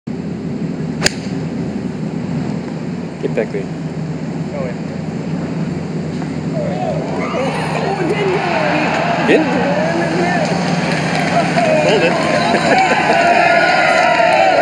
Garcia: Ask and you shall receive 🗣
In the first round of the 2021 TOUR Championship, Sergio Garcia holes out for eagle after calling it in mid air on the opening par-4 1st hole